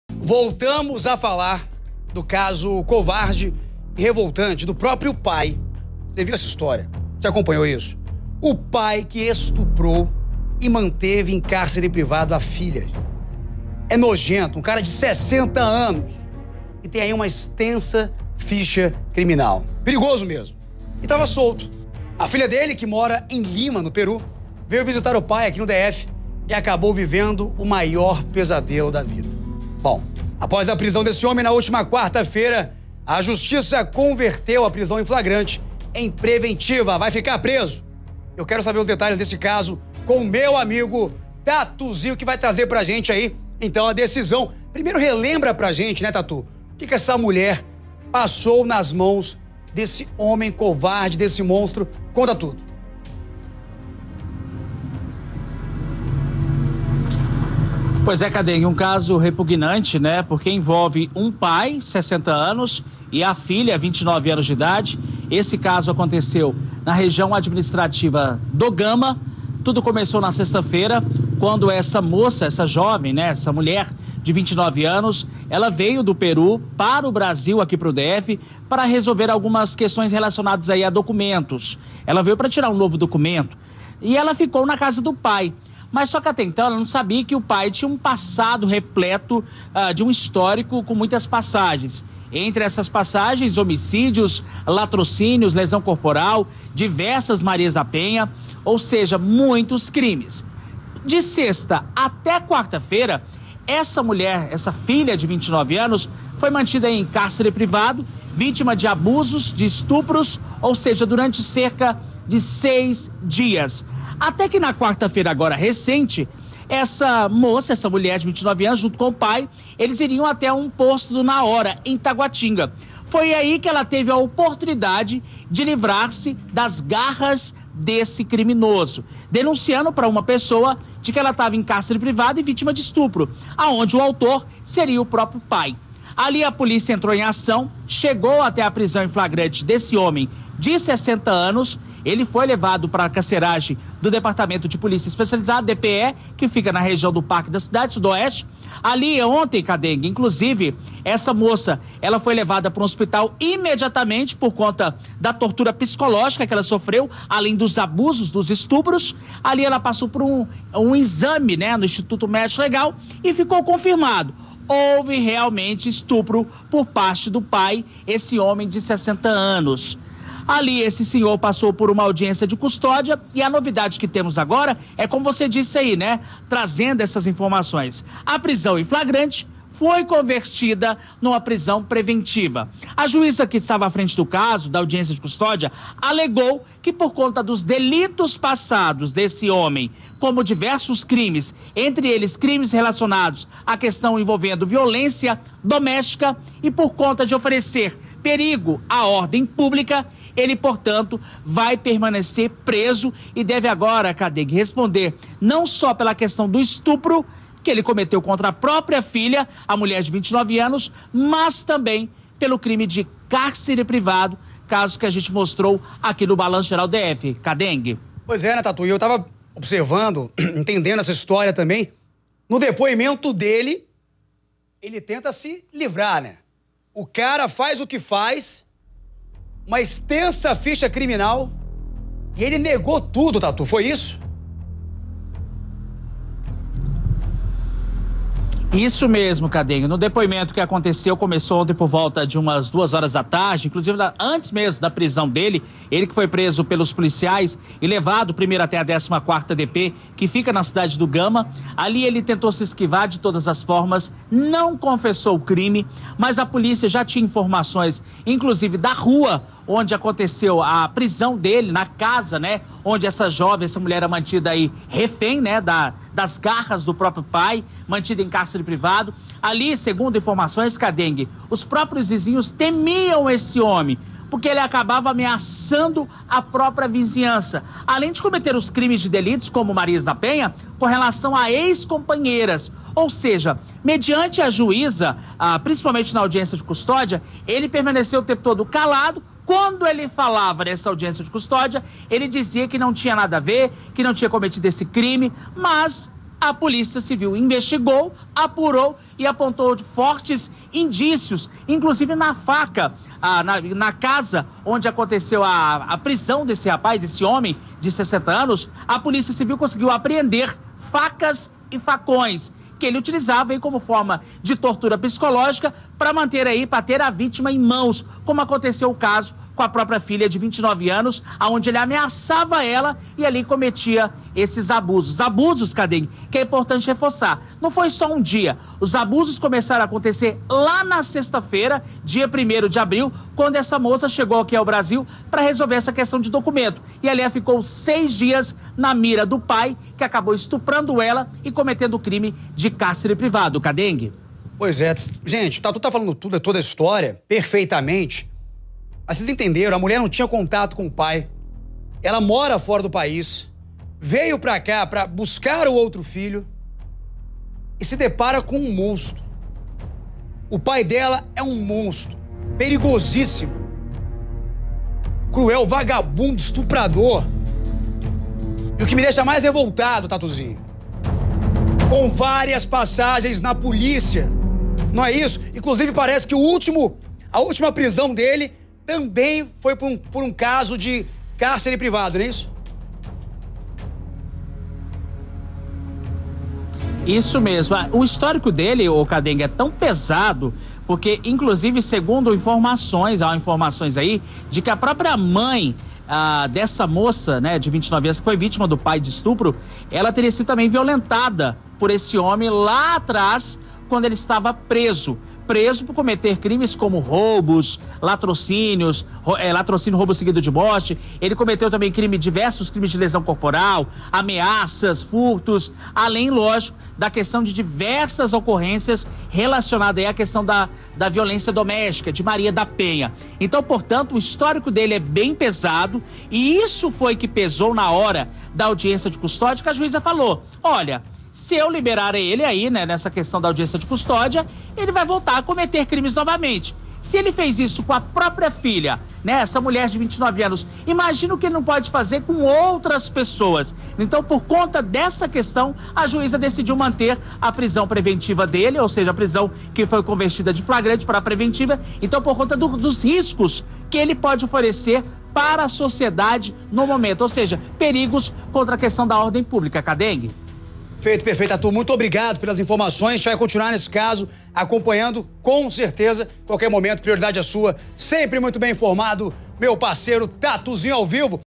Prisão de pai que manteve filha em cárcere privado passa a ser preventiva
A Justiça converteu a prisão em flagrante para preventiva do pai acusado de estuprar e manter a filha em cárcere privado. A peruana mora em Lima e veio visitar o senhor de 60 anos no Distrito Federal. A vítima foi encaminhada ao IML (Instituto de Medicina Legal), onde foi comprovado que realmente houve o abuso sexual.